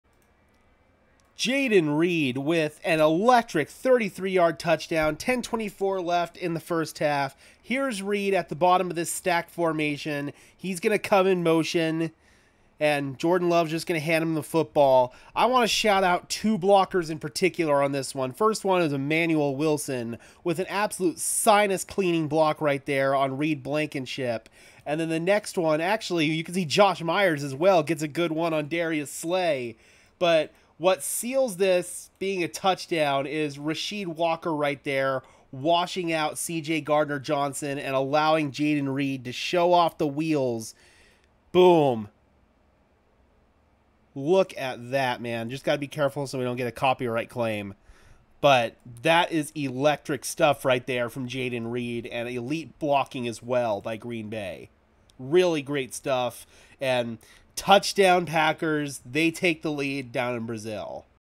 Packers WR Jayden Reed Shows ELECTRIC SPEED on 33-yd TD | Packers vs Philadelphia Eagles
Sparkin' Sports Football Hosted by Gage Madrid